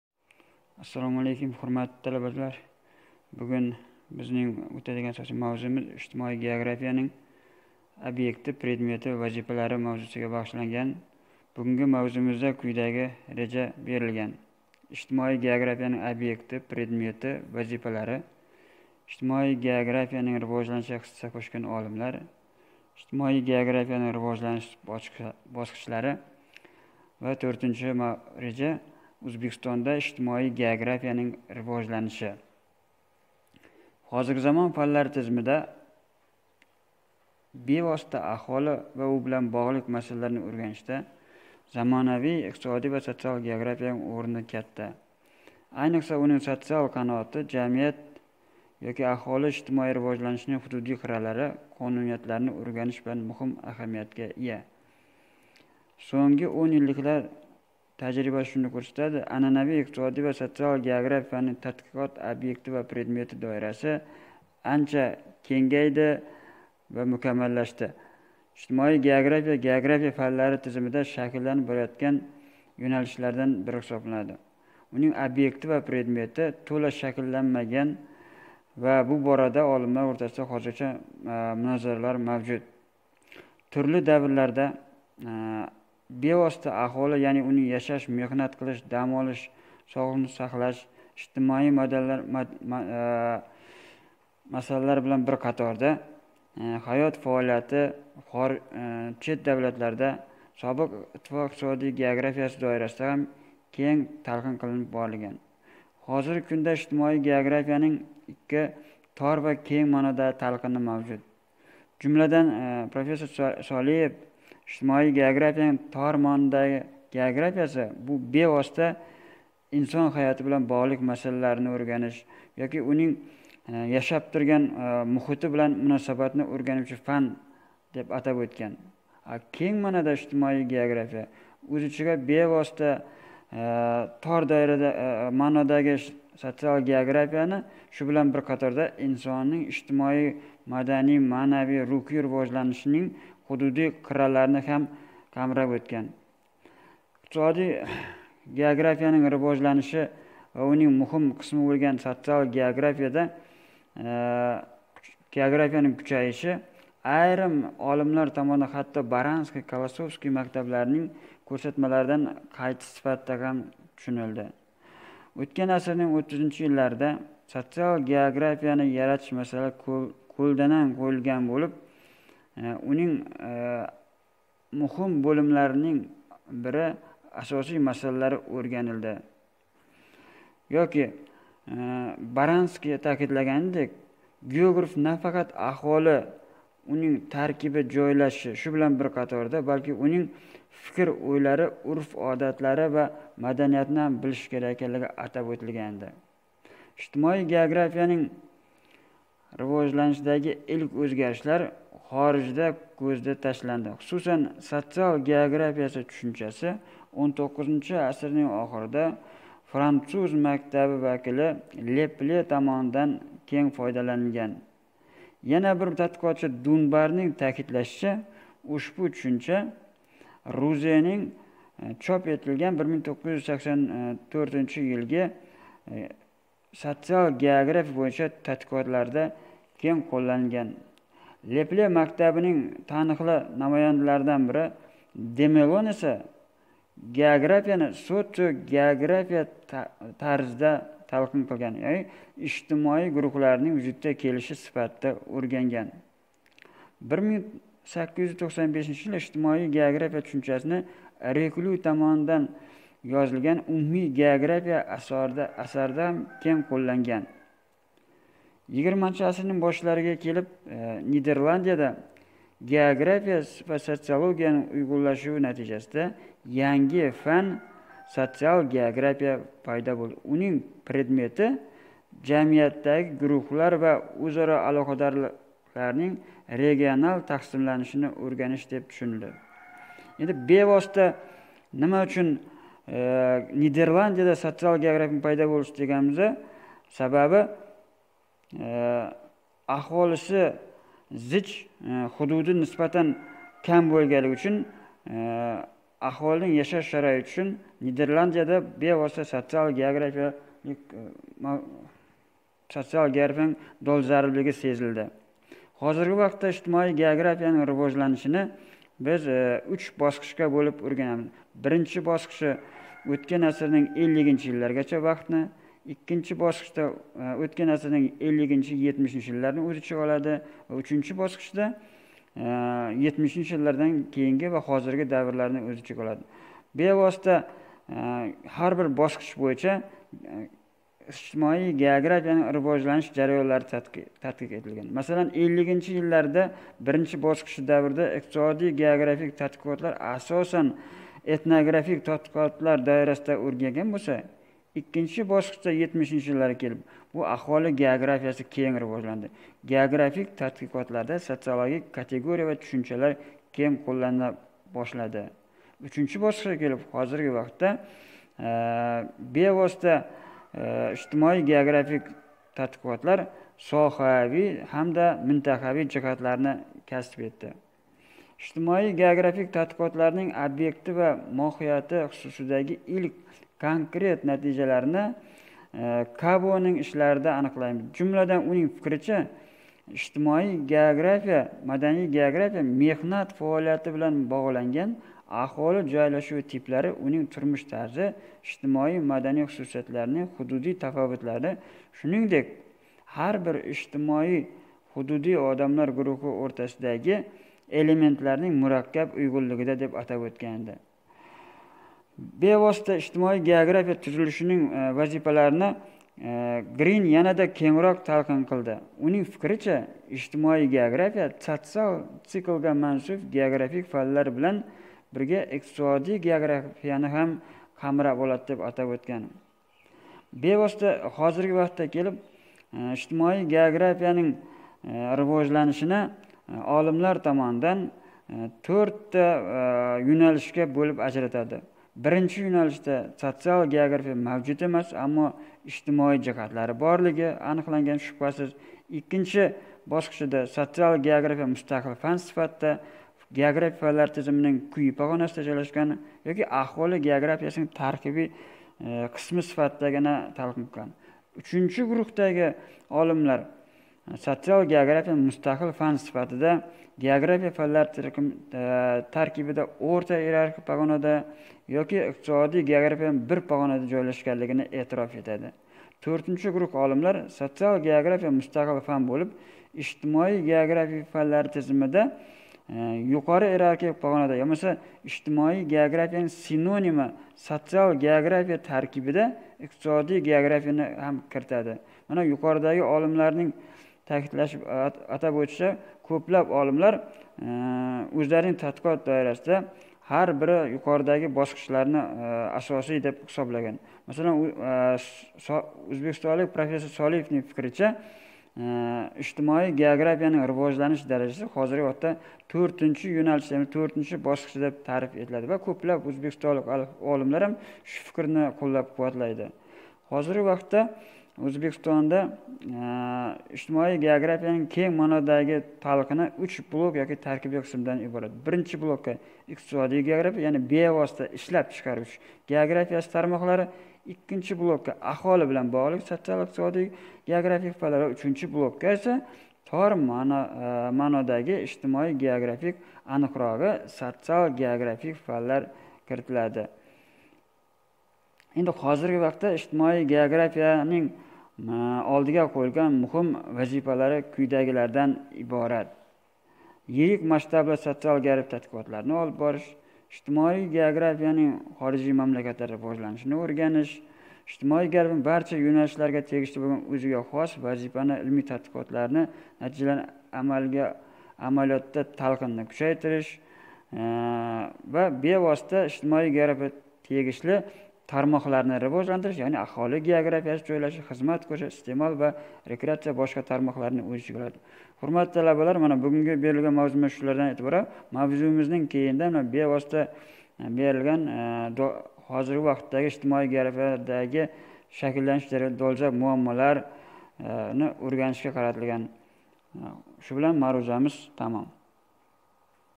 Ijtimoiy geografiyaning obekti, predmeti va vazifalari,
Mavzuda  Ijtimoiy geografiyaning obekti, predmeti va vazifalari, uning rivojlanishiga hissa qushgan olimlar, rivojlanish bosqichlari, O’zbekistonda ijtimoiy geografiyaning rivojlanishi kurib chiqiladi.